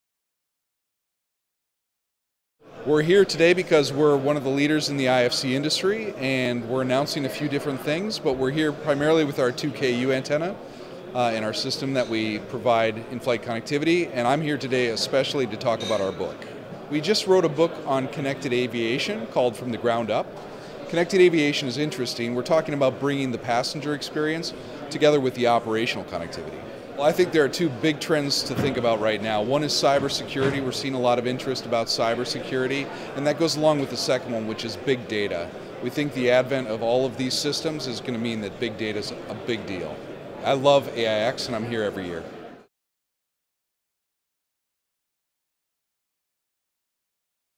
Interview with Andrew Kemmetmueller of Gogo
Check out our exclusive interview with Andrew Kemmetmueller from Gogo, where we delve into the world of inflight connectivity and passenger experiences. Discover how Gogo is leading the way in providing innovative solutions to keep passengers connected and entertained while soaring at 30,000 feet. 📡✈️